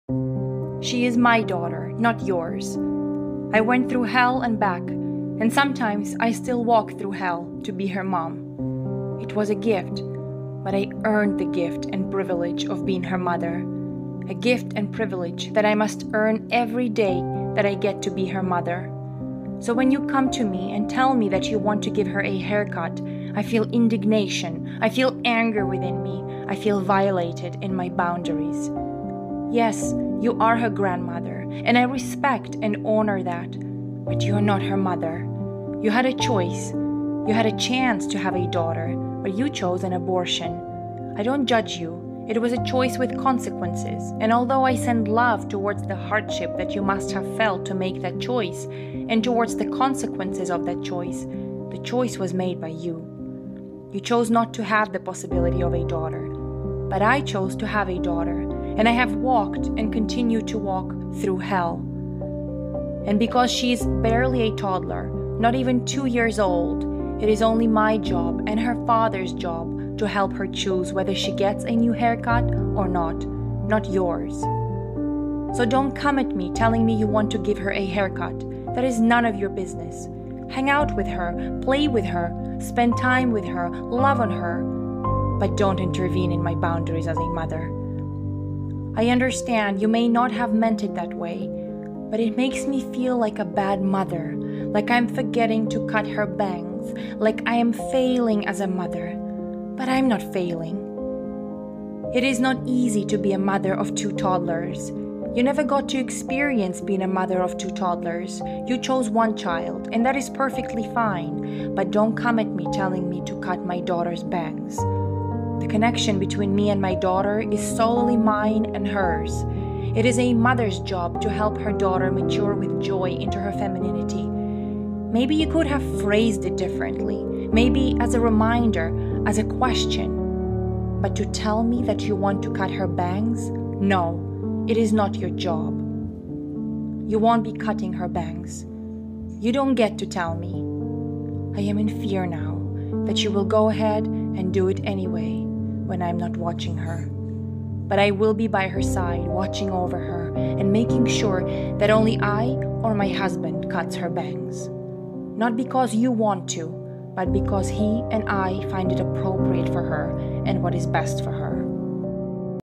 Defending My Motherhood | Poem by Daria Hsu
In this deeply personal and emotional poem, "Defending My Motherhood," I express the raw feelings of anger, powerlessness, and frustration I experienced when my mother-in-law suggested cutting my one-year-old daughter's hair. While I acknowledge that my words may seem harsh, especially mentioning something as sensitive as abortion, this poem reflects my honest emotions in that moment. I believe in honoring my feelings, even when they are difficult to confront. Join me as I share this candid piece of my journey as a mother.